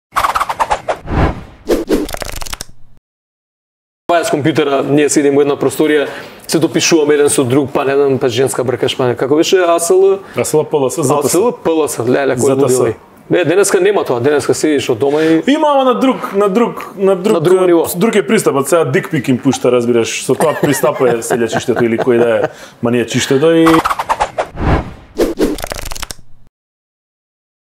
Вербалиум Водкаст - Гораст За Модерното Додворување На Сељачишта Со Дик Пикс
Премиерно Издание на Вербалиум со Гораст Цветковски